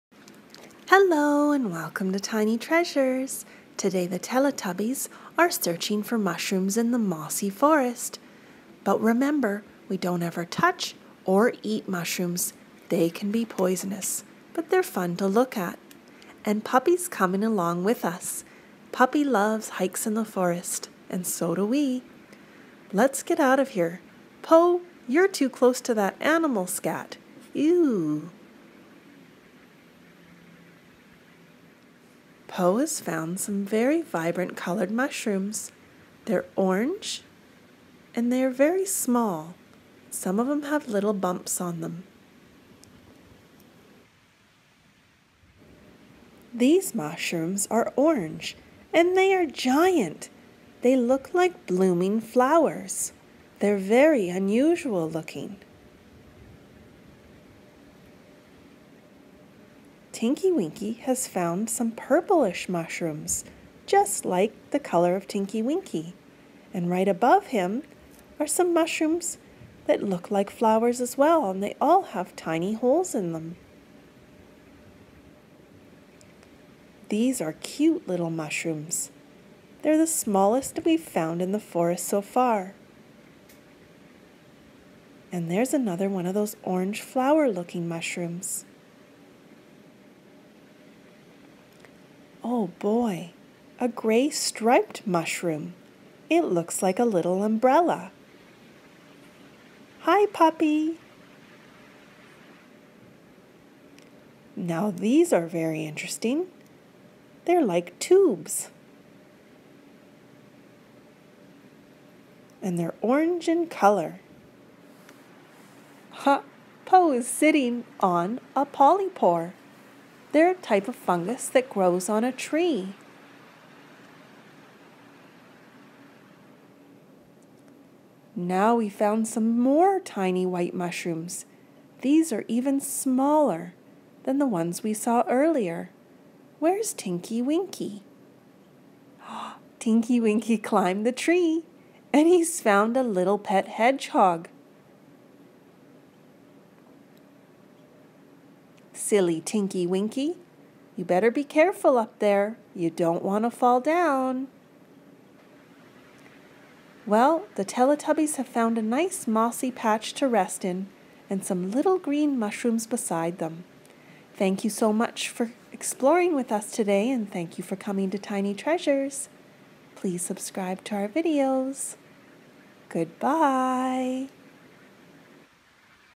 TELETUBBIES TOYS Mushroom Forest Adventure!
Tiny Treasures explores the mossy forest for colorful unusual mushrooms. Enjoy!